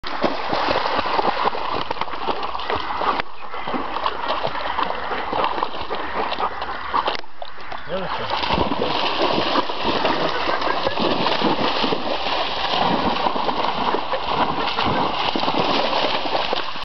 ridgebacks Yeke Yeke Annakiya "Kiya" & Bura - playing in the water (2007 - June - 10)
two ridgebacks sisters playing in the water. more Annakiya & Bura pictures you can find